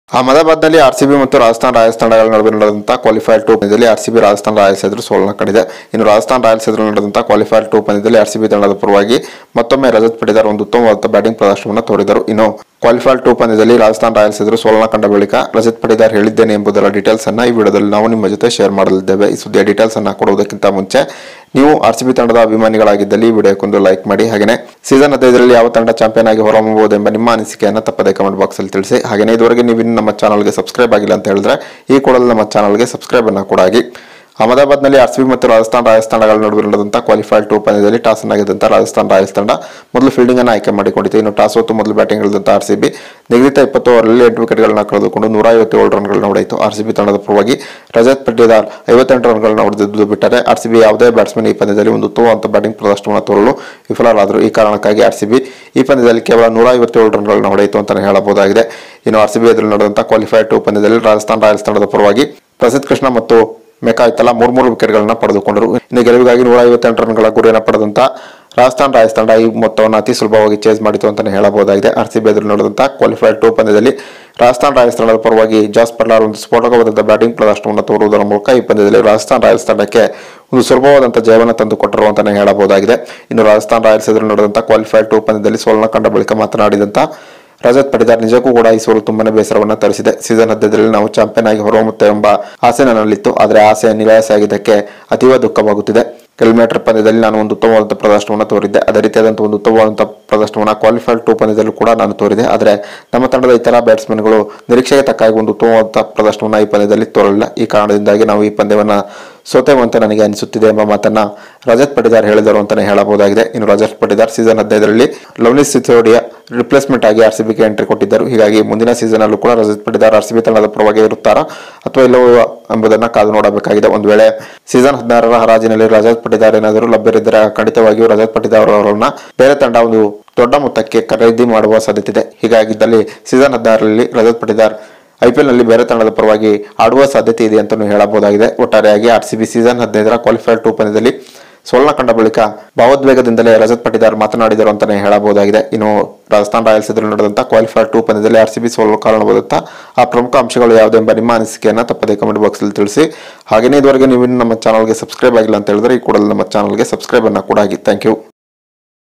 ಮುಂದಿನ ಸೀಸನ್ ನಲ್ಲಿ ಬೇರೆ ಟೀಂ ಗೆ ಹೊಗ್ತೀನಿ.!! ಸೋಲಿನ ನಂತರ ಪಟಿದಾರ್ ಹೇಳಿದ್ದೇನು | RCB | Virat Kohli
Images Courtesy by Star Sports and BCCI / IPL